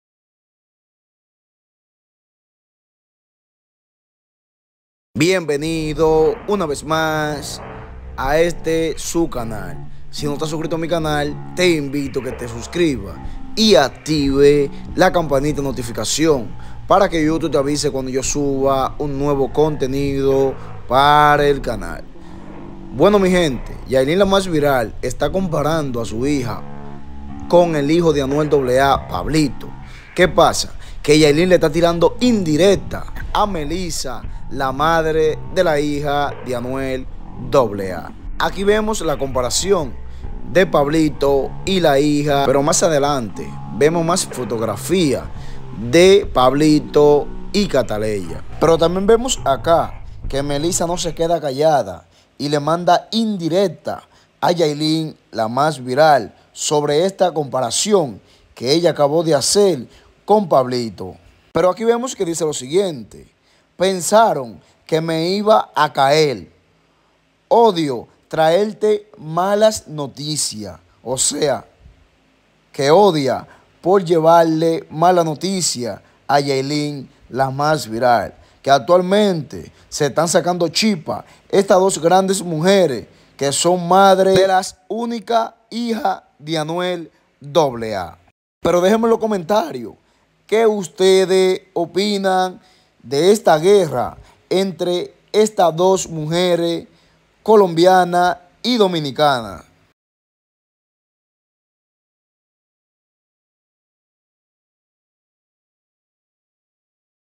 YAILIN MUESTRA SU HIJA . MELISSA LE TAPA LA BOCA A YAILIN POR ANUEL 😲
YAILIN MUESTRA SU HIJA . MELISSA LE TAPA LA BOCA A YAILIN 😲